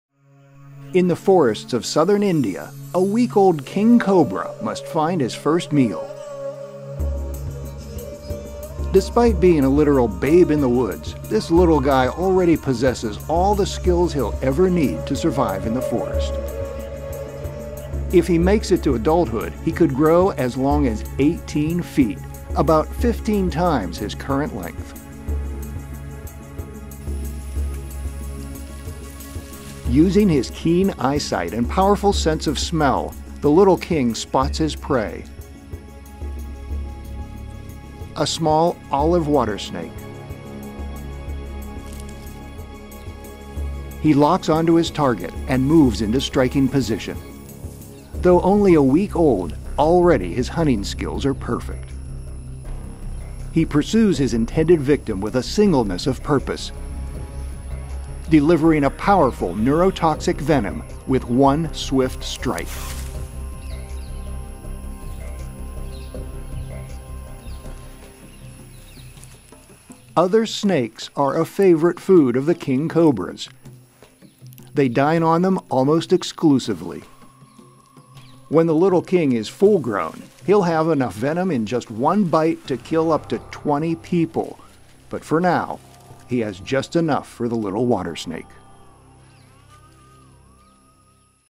King Cobra Devours Water Snake | National Geographic
At a week old, this baby king cobra already has the hunting skills of an adult.